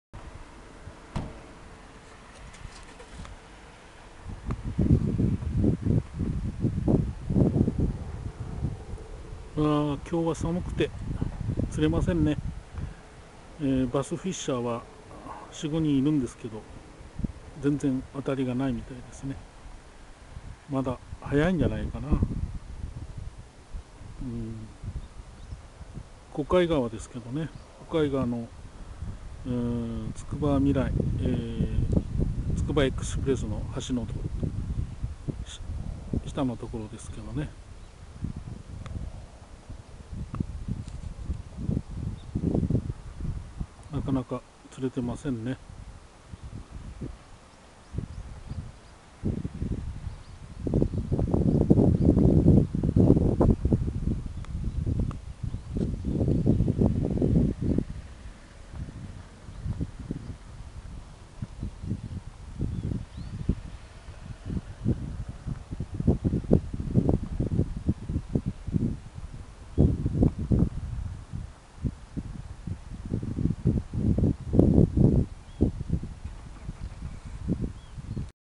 筑波虎
説明2016年3月14日　小貝川、ブラックバスまだ動かない。ワームでも食ってこない。3月末には堰が閉められ水があふれる、その時は活性化する。